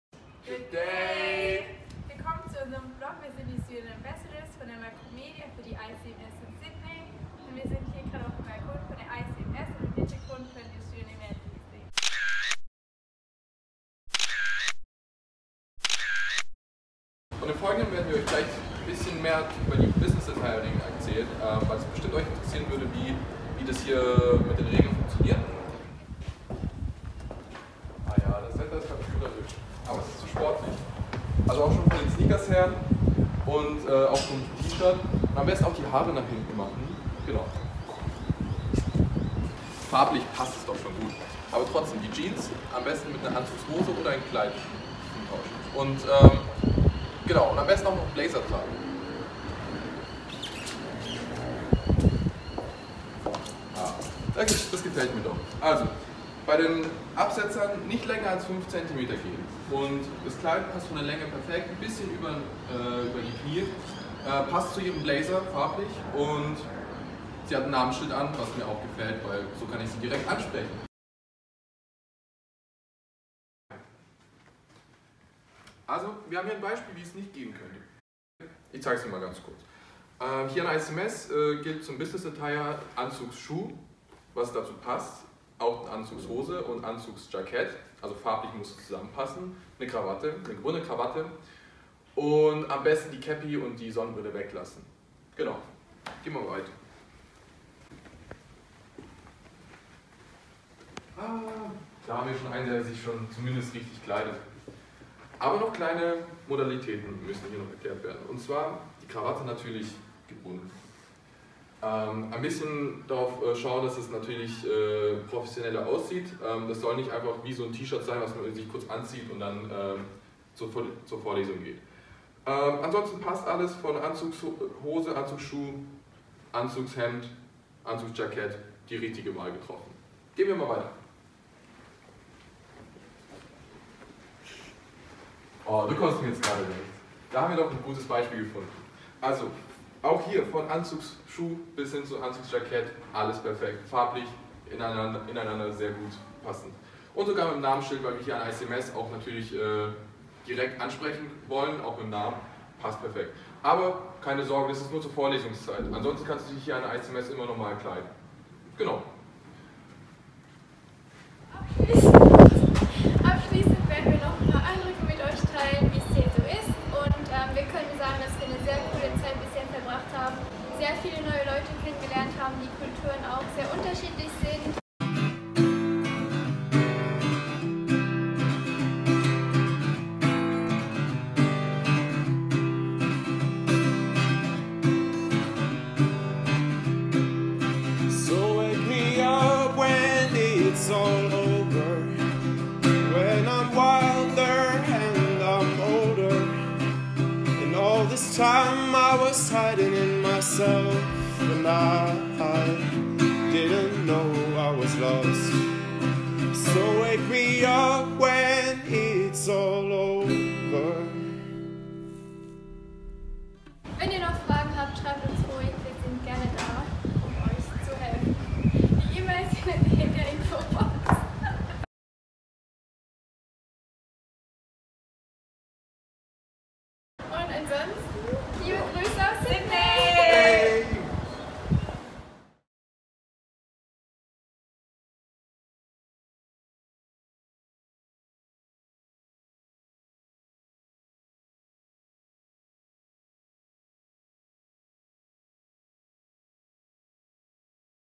Der Macromedia 6+1 Vlog 2017: Caroline, Kim und Denis aus Sydney / No. 8
Ob Peking oder Paris, Buenos Aires oder Bangkok: Ihr drittes Semester verbringen die Bachelor-Studierenden der Hochschule Macromedia an einer von über 20 Partnerhochschulen weltweit.

In diesem Video berichten die Management-Studierenden Caroline, Kim und Denis von ihrer Zeit - und der Kleiderordnung - am International College of Management (ICMS) in Sydney  (Australien).

Weitere studentische Erfahrungsberichte aus dem Macromedia-Auslandssemester gibt es im 6+1 Blog

Let it be Bejing or Buenos Aires, Seoul or San Diego: Within their BA-study programmes, Macromedia students spend the third semester abroad at one of more than 20 universities worldwide.

In this videoclip management-students Caroline, Kim and Denis tell us again about their experiences - and the dress code - at the International College of Management (ICMS) in Sydney (Australia).

Check out the 6+1 Blog to see what other Macromedia students contribute from their semester abroad